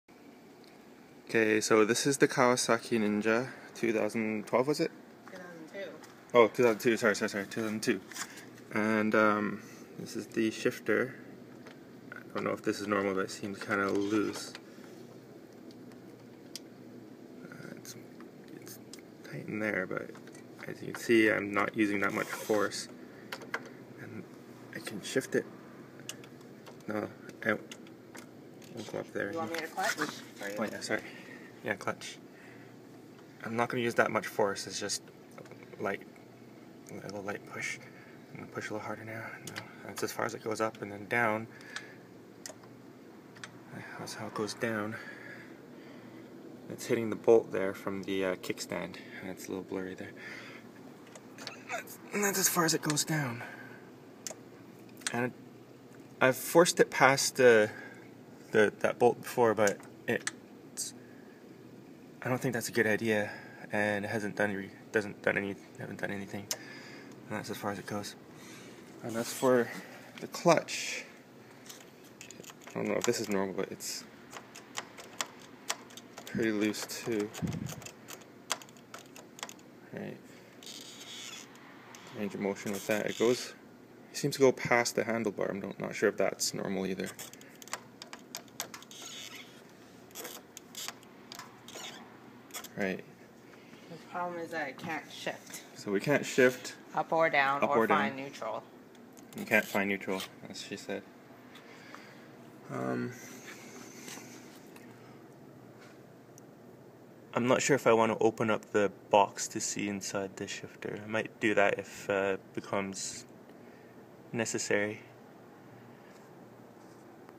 Motorcycle gear lever moves and clicks but can't shift
Hi all, I am a brand new rider and got a 2002 Kawasaki ninja 250r that I've ridden for one week. It runs fine (after needing an impossibly long warm up every morning, like 5-10 mins minimum). Anyway, today I thought that the gear lever felt strange. I was able to get up to the second gear and it got stuck. Even though I can move the gear lever up or down, and it even clicks, but it can't shift anymore (doesn't make the almost clunking sound of shifting). When I force the gear lever down with my foot, it actually gets stuck on the bolt that holds the kick stand in place, which it has never done before. Can anyone help me? Thank you.